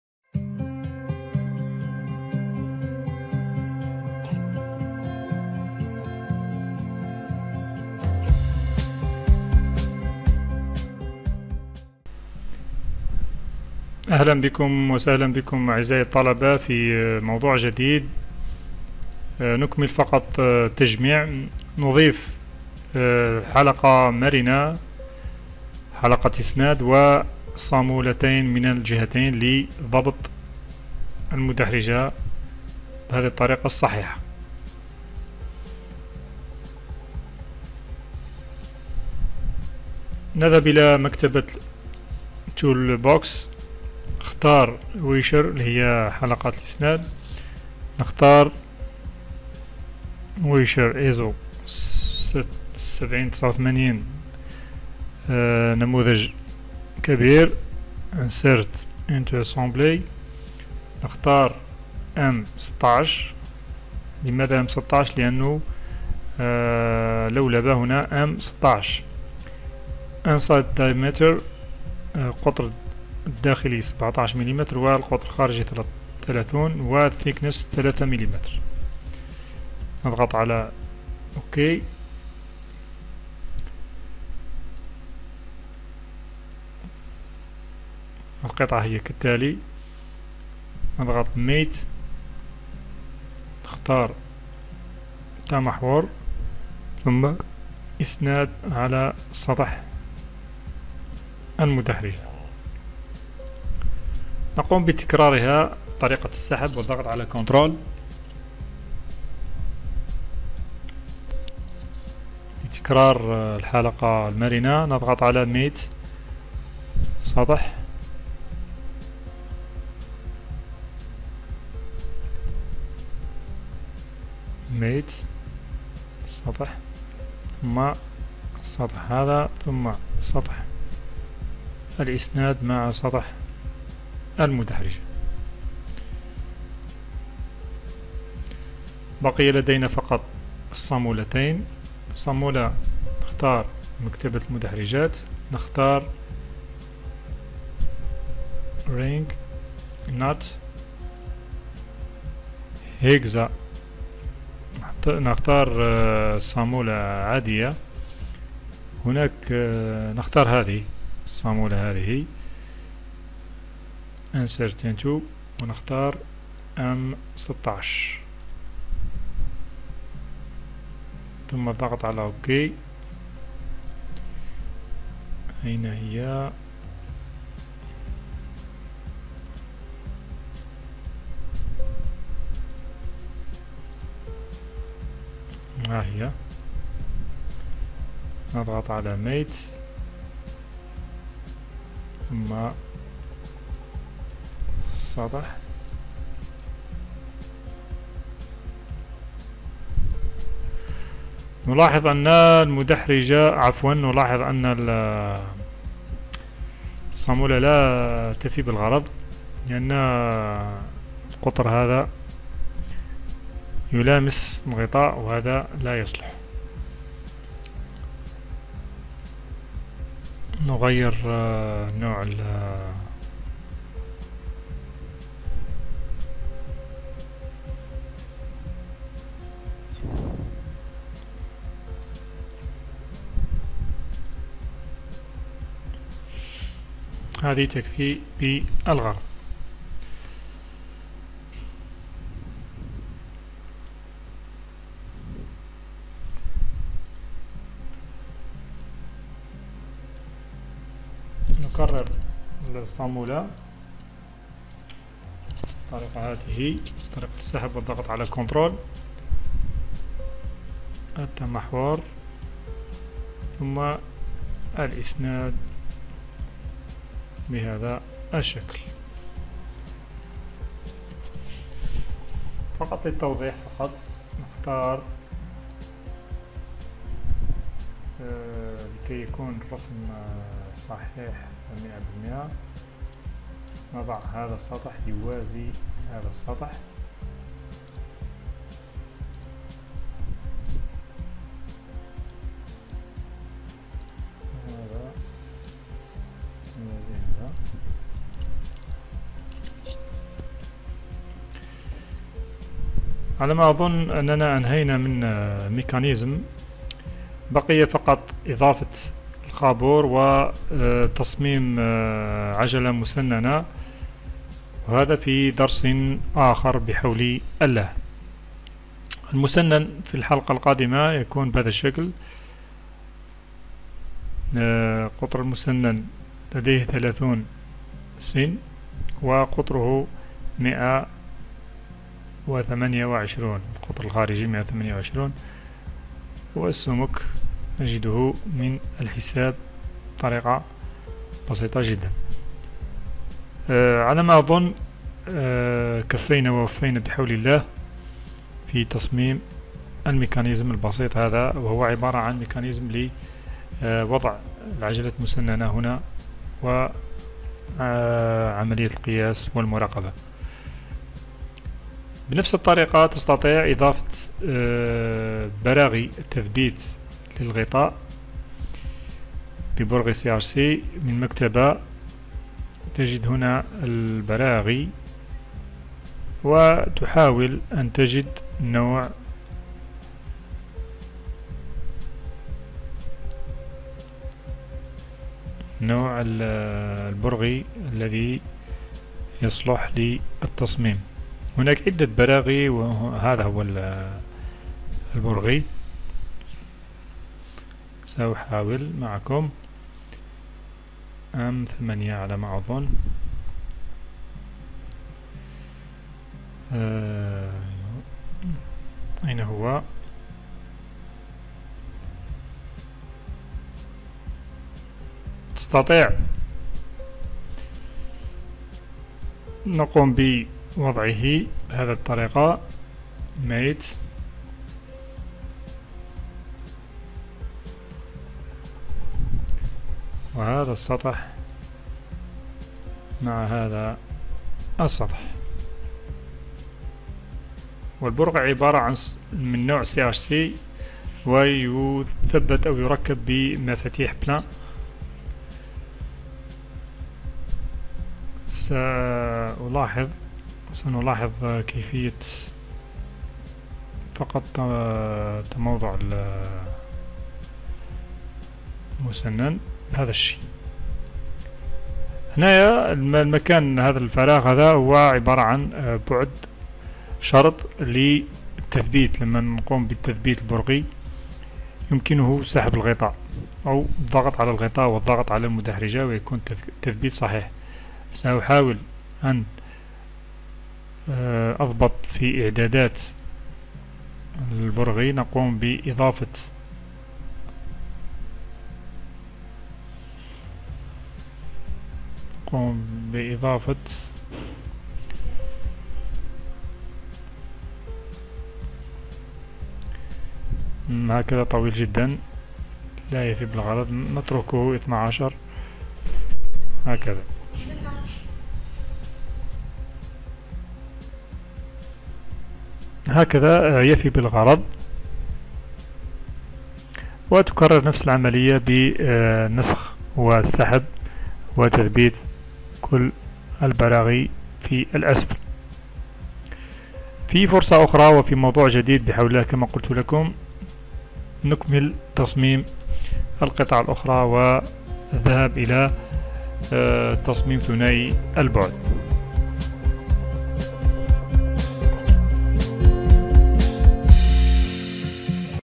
partie 6 - تركيب حلقة إسناد مع الصامولة
درس تطبيقي في كيفية التعامل مع برمجية Solidworks وتصميم مشروع بسيط انطلاقا من ملف مرسوم باليد وتوضيحه بالبرمجية والحصول على ملف تقني كامل من إنشاء جميع القطع ثم التجميع ثم الثنائي البعد أرجو منكم الإستفادة و التفاعل معنا